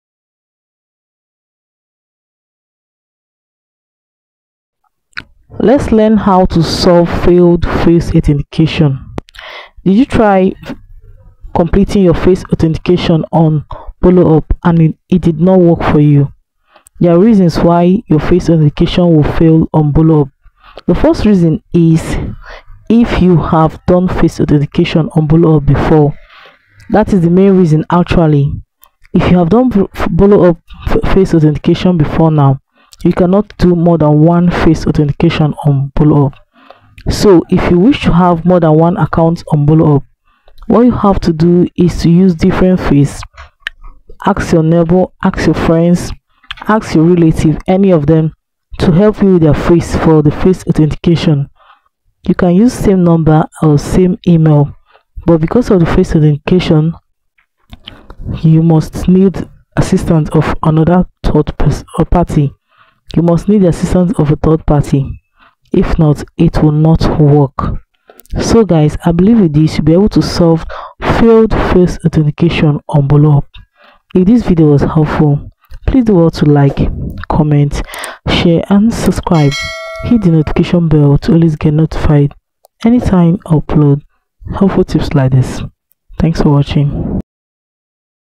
How to Solve Failed Face Authentication on Boloup
Learn More About Boloup Livestream App Here